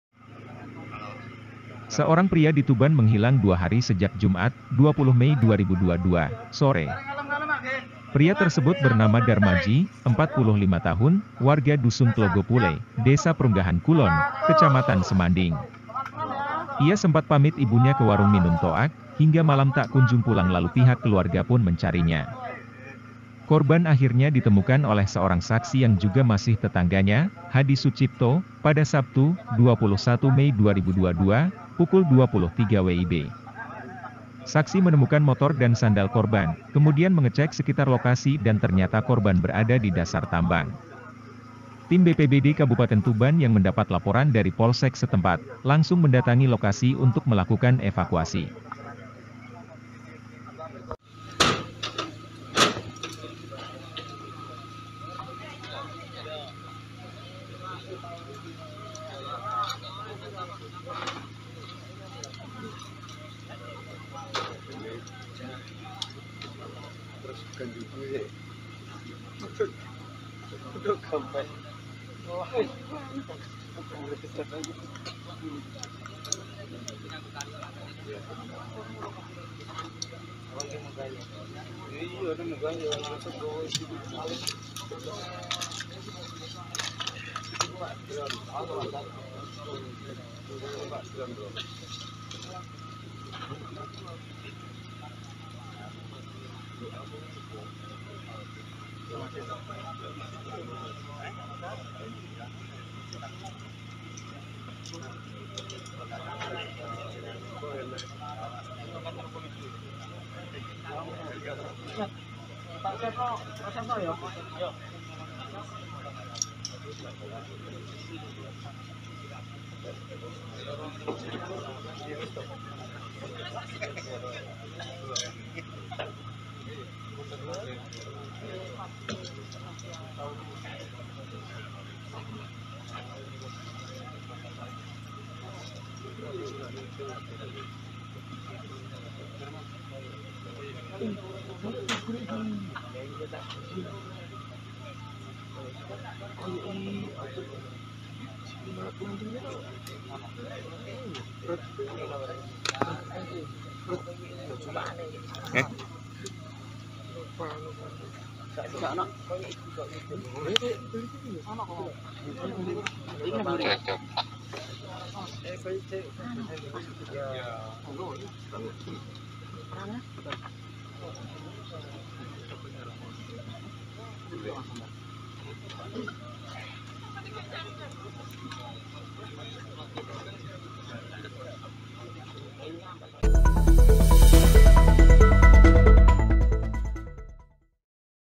Menghilang Dua Hari, Pria di Tuban Ditemukan Meninggal Jatuh 45 Meter di Tambang Kapur
TUBAN - SURYAMALANG.COM
Reporter : M Sudarsono - Editor Video : Yusuf

Seorang pria di Tuban menghilang dua hari sejak Jumat (20/5/2022), sore.

Pria tersebut bernama Darmaji (45), warga Dusun Tlogopule, Desa Prunggahan Kulon, Kecamatan Semanding.

Ia sempat pamit ibunya ke warung minum toak, hingga malam tak kunjung pulang lalu pihak keluargapun mencarinya.

Korban akhirnya ditemukan oleh seorang saksi yang juga masih tetangganya, Hadi Sucipto, pada Sabtu (21/5/2022), pukul 23.00 WIB.

Saksi menemukan motor dan sandal korban, kemudian mengecek sekitar lokasi dan ternyata korban berada di dasar tambang.

Tim BPBD Kabupaten Tuban yang mendapat laporan dari Polsek setempat, langsung mendatangi lokasi untuk melakukan evakuasi.

Korban ditemukan jatuh dari ketinggian 45 meter, di bekas galian tambang batu kapur di desa setempat, Minggu (22/5/2022), pukul 00.15 WIB.

"Kita mendapat laporan ada orang jatuh dalam kondisi meninggal, ketinggian 45 meter," kata Kepala Pelaksana BPBD Kabupaten Tuban, Sudarmaji.

Ia menjelaskan, tim rescue yang mendapat laporan langsung melakukan pemetaan untuk menyelamatkan korban yang berada di jurang.

Tim selanjutnya turun ke dasar bekas tambang kemudian mengangkat korban, setelah itu diserahkan ke pihak keluarga.

Dari hasil pemeriksaaan medis, tidak ditemukan adanya tanda-tanda kekerasan.

Korban mengalami luka terbuka di bagian kepala dan lecet di sejumlah badan.

"Pihak keluarga menerima kejadian tersebut dan tidak meminta autopsi," pungkasnya